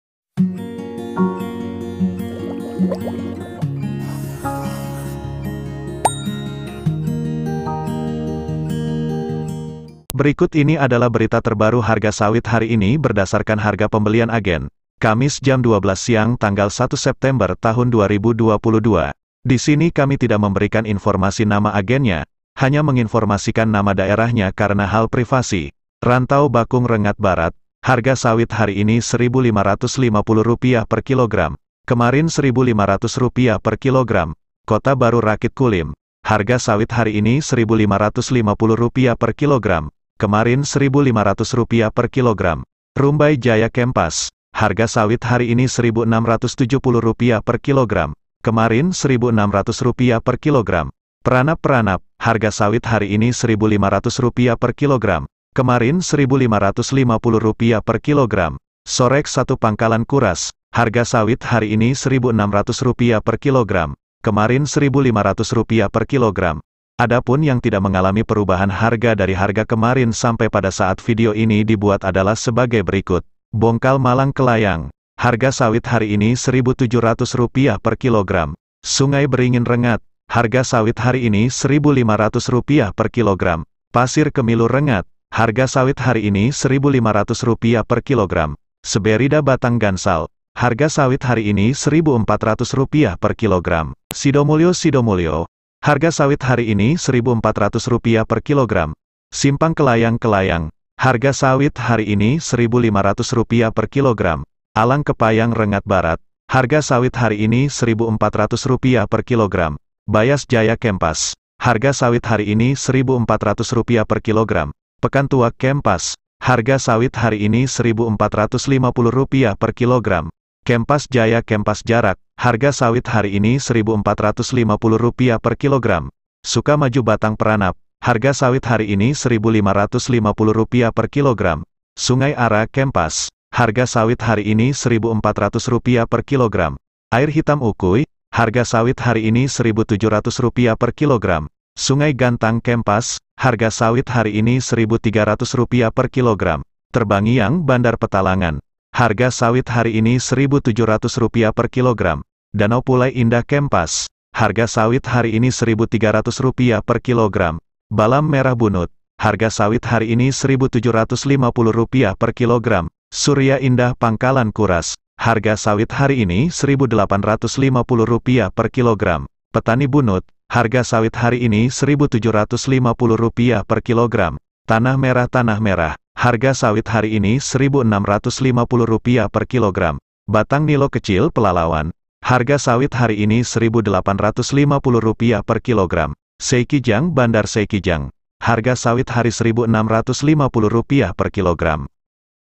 Harga sawit pembelian Agen terbaru 1 September 2022 - Berita terbaru harga sawit hari ini
Berikut Ini Adalah Berita terbaru harga sawit hari ini Berdasarkan Harga Pembelian agen, Kamis jam 12 siang 1 September 2022. Disini kami Tidak Memberikan Informasi Nama agennya, Hanya Menginformasikan Nama Daerahnya Karena Hal Privasi.

Rantau Bakung Rengat Barat.
Harga sawit hari ini Rp 1.550 Per Kilogram.
Kemarin Rp 1.500 Per Kilogram.

Kota Baru Rakit Kulim.

Rumbai Jaya Kempas.
Harga Sawit Hari Ini Rp 1.670 Per Kilogram.
Kemarin Rp 1.600 Per Kilogram.

Peranap Peranap.
Harga sawit hari ini Rp 1.500 Per Kilogram.
Kemarin Rp 1.550 Per Kilogram.

Sorek Satu Pangkalan Kuras.
Harga Sawit Hari Ini Rp 1.600 Per Kilogram.

Adapun yang tidak mengalami perubahan harga dari harga kemarin sampai pada saat video ini di buat adalah sebagai berikut.

Bongkal Malang Kelayang.

Sungai Beringin Rengat.

Pasir Kemilu Rengat.

Seberida Batang Gansal.

Sidomulyo Sidomulyo.

Simpang Kelayang Kelayang.

Alang Kepayang Rengat Barat.

Bayas Jaya Kempas.

Pekan Tua Kempas.

Kempas Jaya Kempas Jarak.

Suka Maju Batang Peranap.

Sungai Ara Kempas.

Air Hitam Ukui.

Sungai Gantang Kempas.

Terbangiang Bandar Petalangan.

Danau Pulai Indah Kempas.

Balam Merah Bunut.

Surya Indah Pangkalan Kuras.

Petani Bunut.

Tanah Merah Tanah Merah.

Batang Nilo Kecil Pelalawan.

Seikijang Bandar Seikijang.
Harga Sawit Hari Rp 1.650 Per Kilogram.